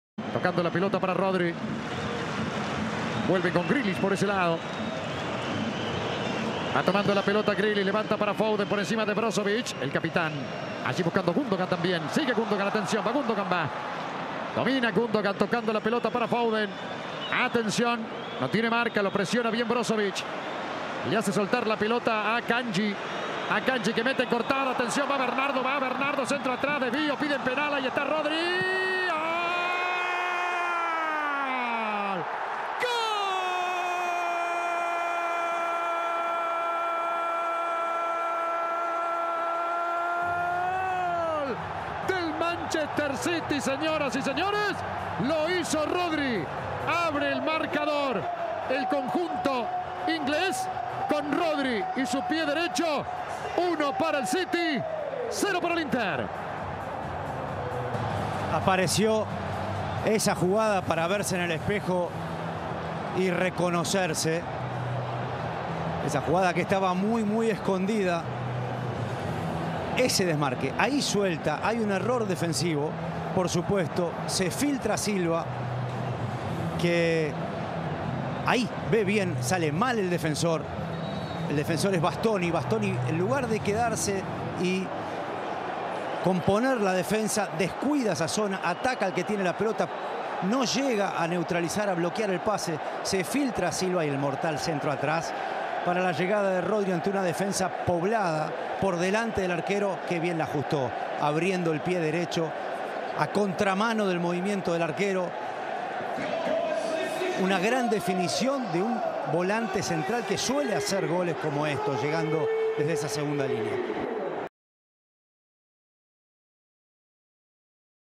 Manchester City 1 Inter 0 - Champions 2022/23 (por Mariano Closs)
El gol del partido, por Mariano Closs y Diego Fernando Latorre para ESPN/FOX Sports.